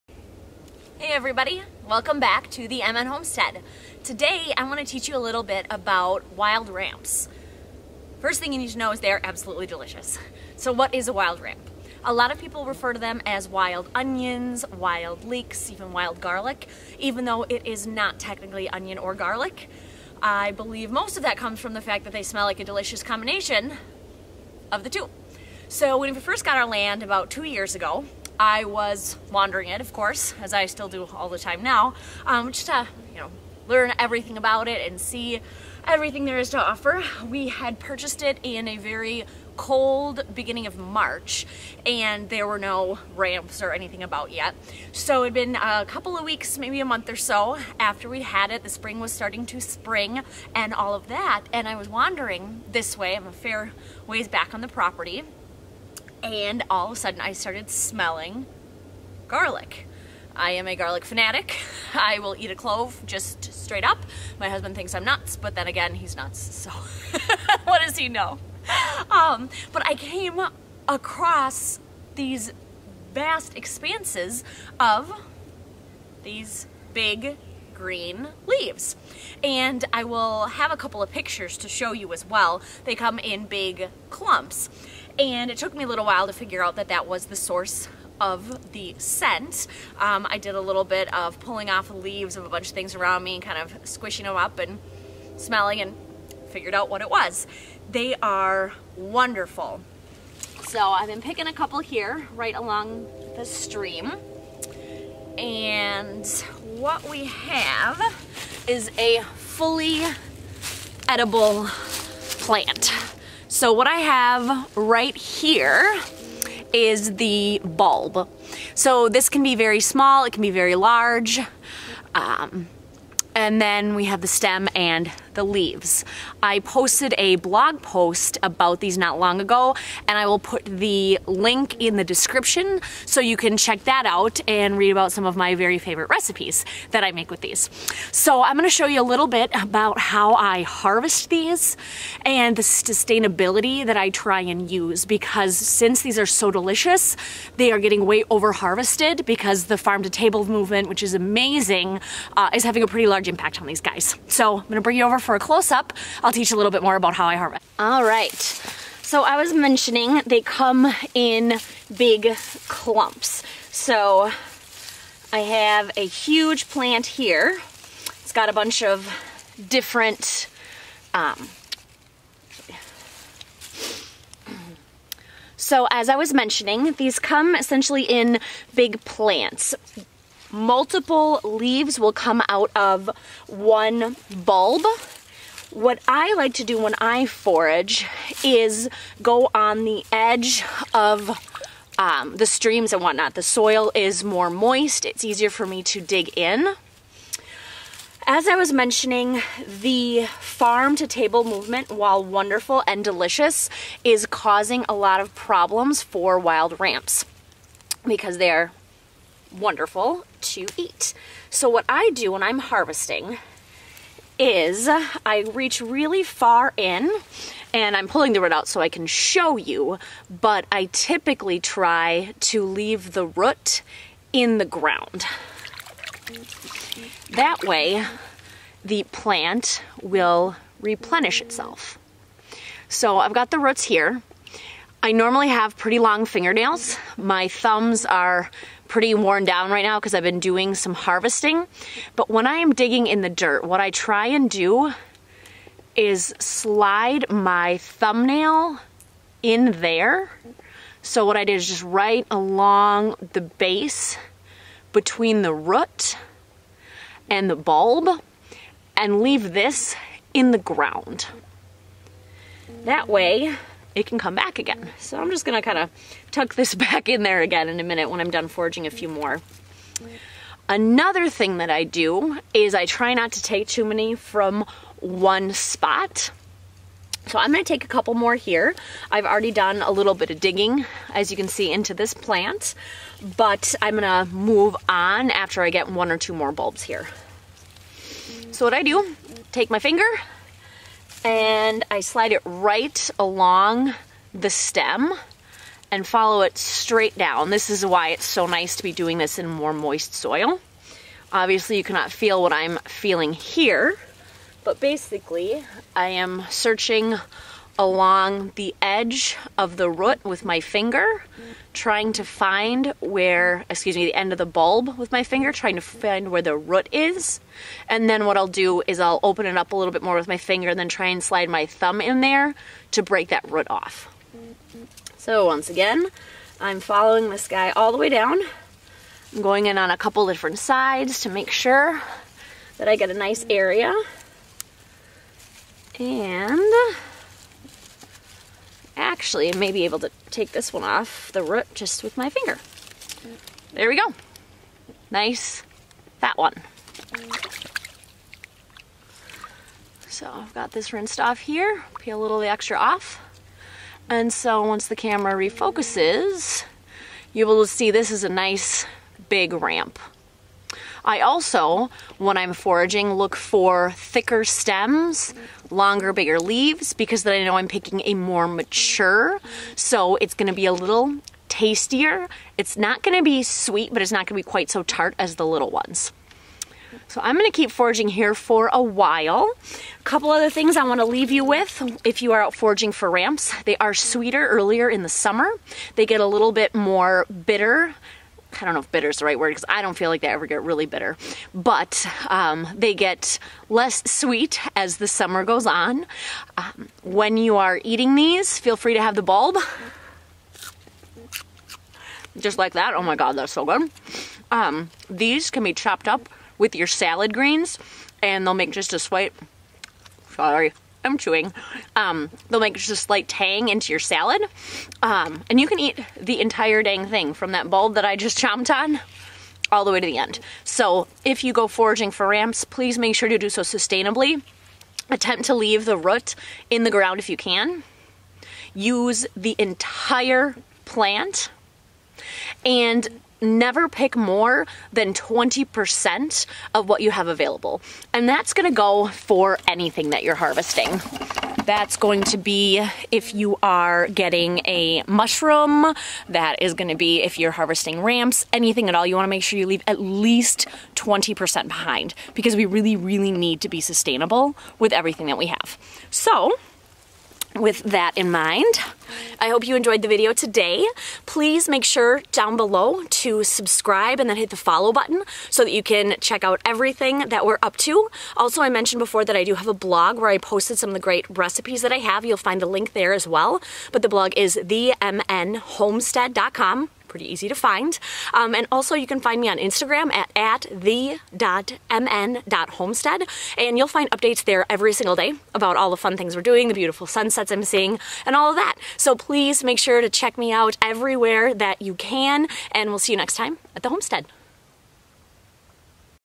foraging for wild ramps
I love wild ramps, they’re delicious and enjoyable to forage. Learn about how I use sustainable foraging methods to harvest, and check out my wild ramp blog post at themnhomestead.com for some of my favorite recipes!